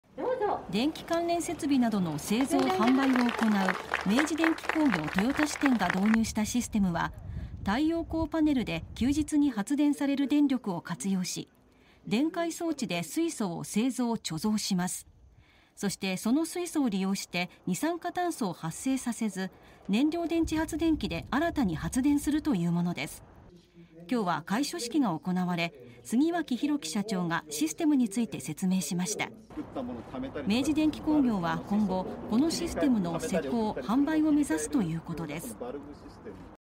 太陽光発電の電力活かし水素製造→燃料電池で新たに発電 愛知県の企業がシステム完成 (24/05/09 16:37)
愛知県内の企業で、太陽光発電の電力を活用して水素を製造し、それを使い新たに発電するシステムが完成しました。

　電気関連設備などの製造・販売を行う明治電機工業豊田支店が導入したシステムは、太陽光パネルで休日に発電される電力を活用し、電解装置で水素を製造・貯蔵します。

　そして、その水素を利用して、二酸化炭素を発生させず燃料電池発電機で新たに発電するというものです。

　9日は開所式が行われ、杉脇弘基社長がシステムについて説明しました。

　明治電機工業は今後、このシステムの施工・販売を目指すということです。